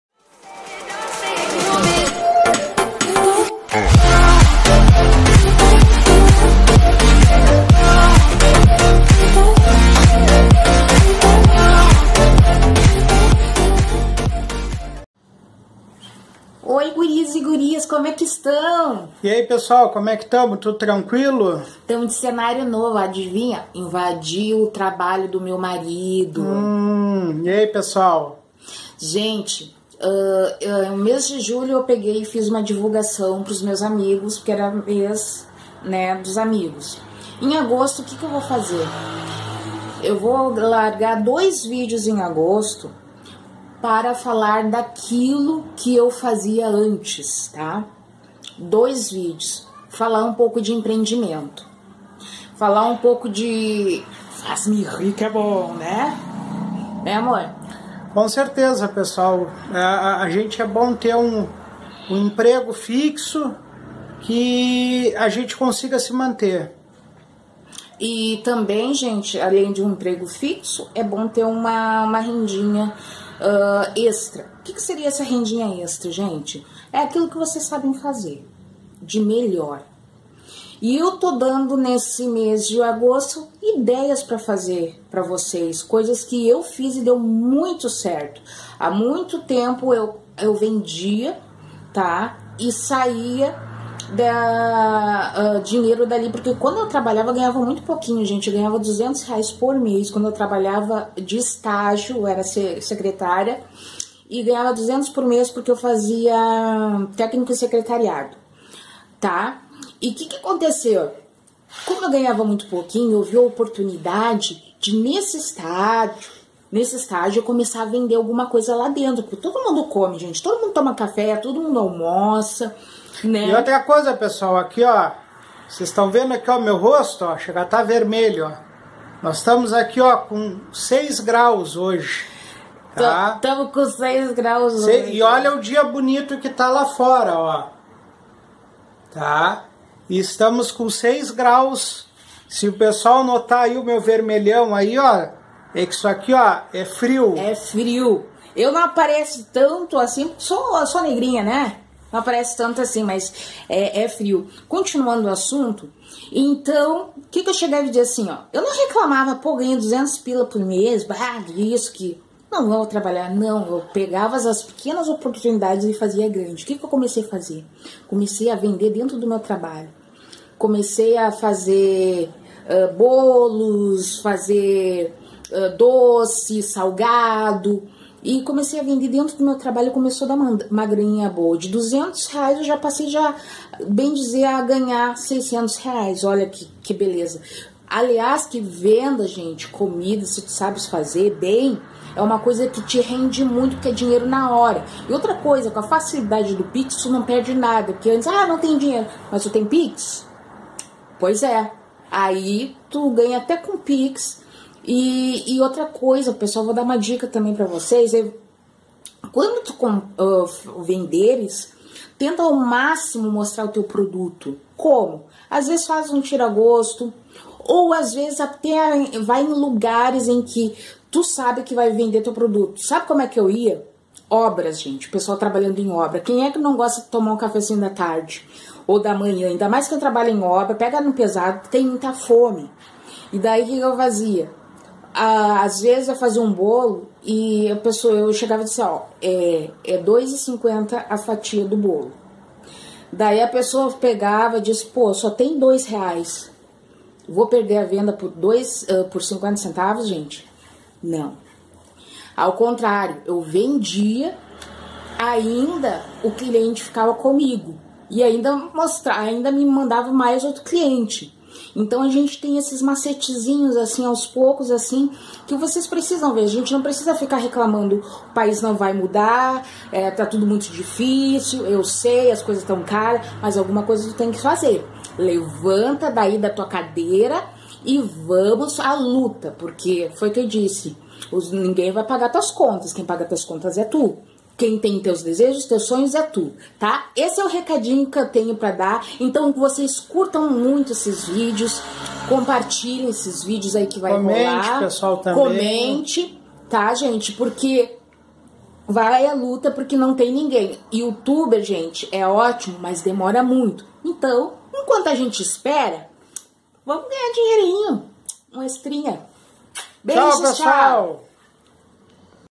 ganhe dinheiro com comida(earn money with food)
neste vídeo irei dar ideias de como fiz renda extra com comida, nos próximos vídeos aguardem que colocarei as receitas.
In this video I will give you ideas on how I made extra income with food, in the next videos wait for me to put the recipes.